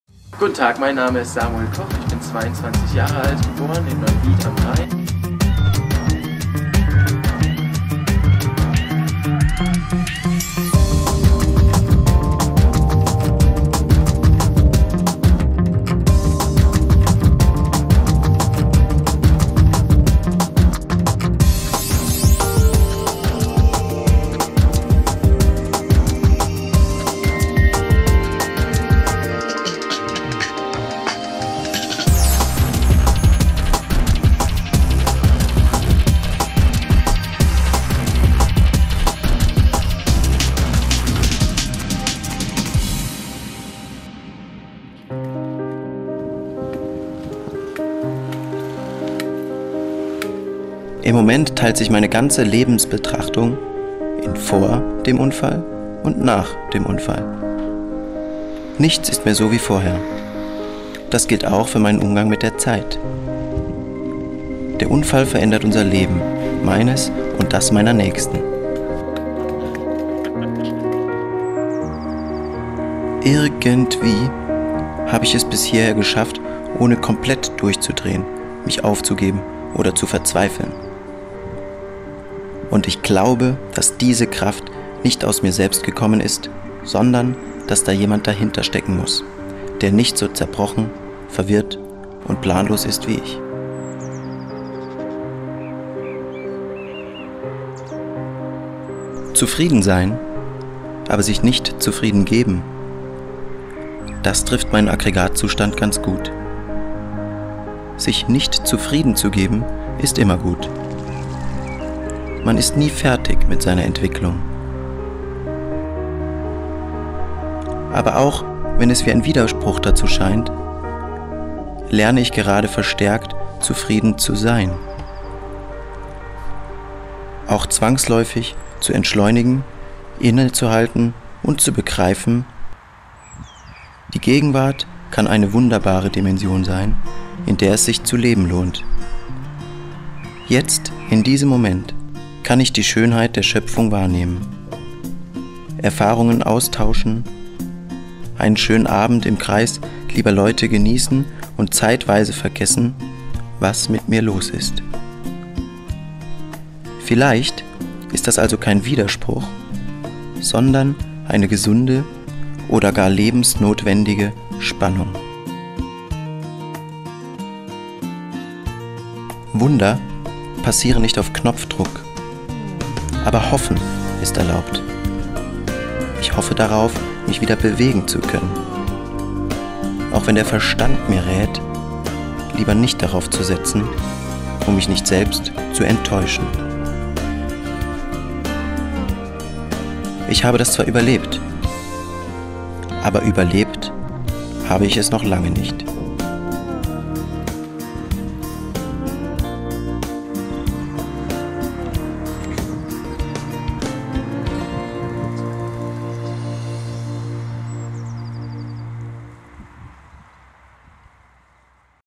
Samuel Koch - Zwei Leben (adeo Verlag)
Samuel Koch sitzt im Rollstuhl. Er ist vom Hals abwärts gelähmt, kann sich kaum bewegen. Aber er kann denken und fühlen. Und er kann hoffen. In seinem Buch "Zwei Leben", das er zusammen mit Christoph Fasel geschrieben hat, erzählt er von seinem Leben vor dem Sprung. Wie er zu "Wetten dass..?" kommt. Davon, wie der Unfall geschieht. Das ist der Augenblick, in dem sein zweites Leben beginnt: Schock, Verzweiflung, Schmerz und Wut. Doch er trifft die Entscheidung, nicht aufzugeben. Und an dem festzuhalten, was ihn trägt. Radikal ehrliche Schilderungen eines jungen Mannes, der nichts mehr zu verlieren hat, sondern nur noch gewinnen kann. Und eine Einladung, die Kostbarkeit des Lebens neu zu schätzen.

Mit einem Vorwort von Thomas Gottschalk und einem Nachwort von Michelle Hunziker.